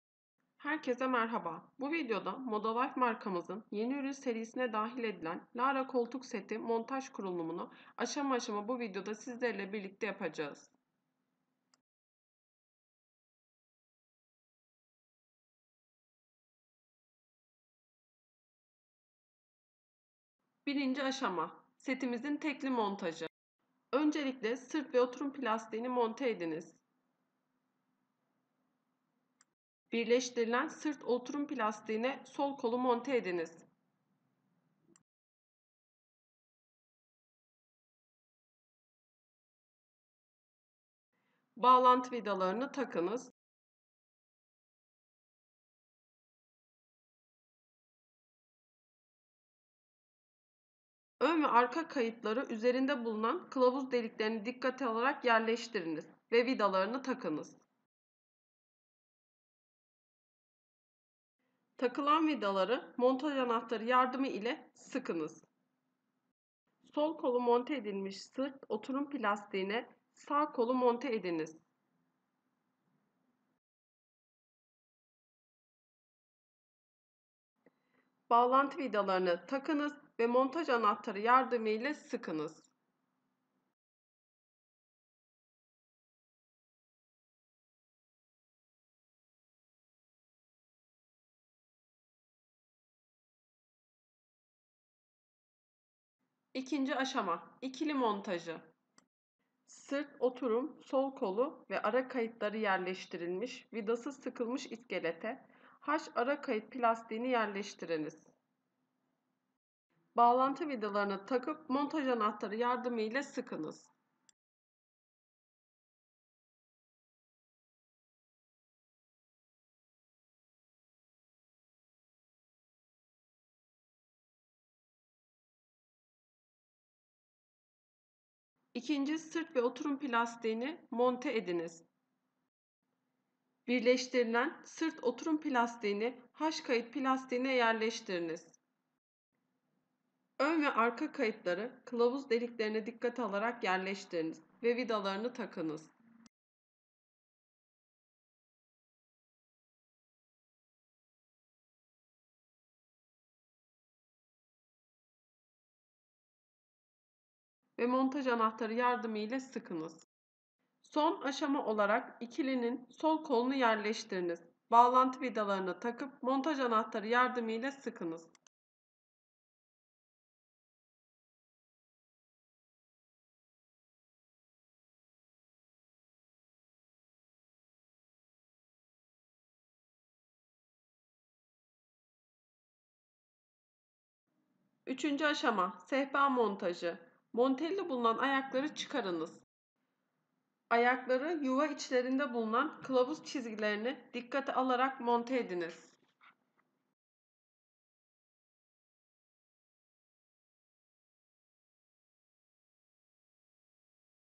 Lara Bahçe Balkon Oturma Grubu Kurulum Montajı
*Lara Oturma Grubu, İkili koltuk, 2 adet tekli koltuk ve sehpadan oluşmaktadır.
*Mutfak, balkon, bahçe, salon vb. gibi iç ve dış mekanlarda kullanılabilir.
*Beyaz, cappucino, antrasit renk seçeneğiyle.
*Minder kumaşı tek renktir.
*Oturma grubu fiyatına minder dahildir.
*Ürünümüz 2 sene üretim ve işçilik hatalarına karşı garantilidir.